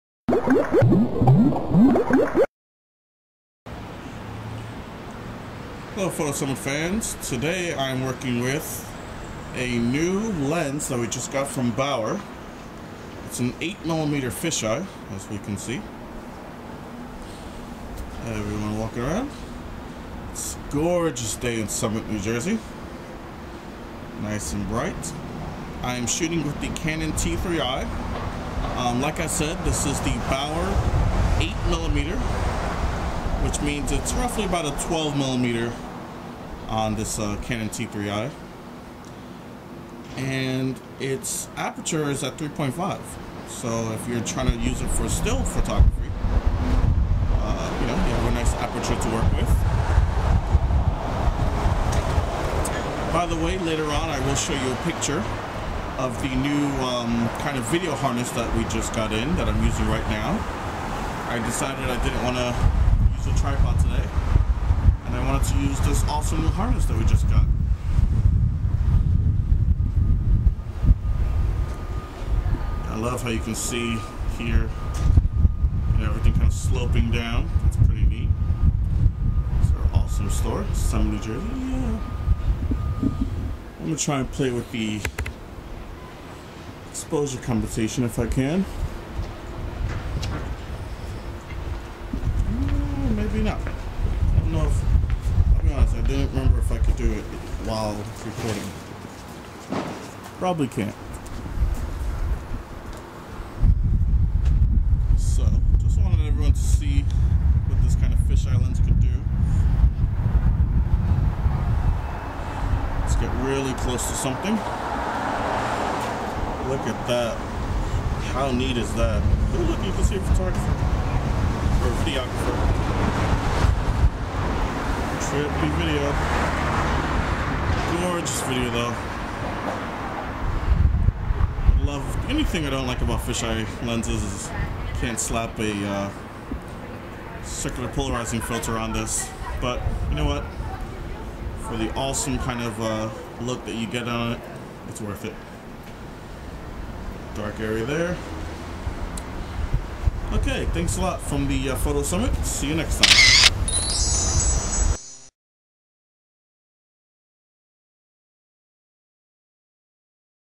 Bower 8mm fisheye
Using the Bower 8mm f3.5 fisheye.  Its fully manual focus but who cares.  its an affordable fisheye under $400.  Available for pentax, Sony, Nikon and Canon.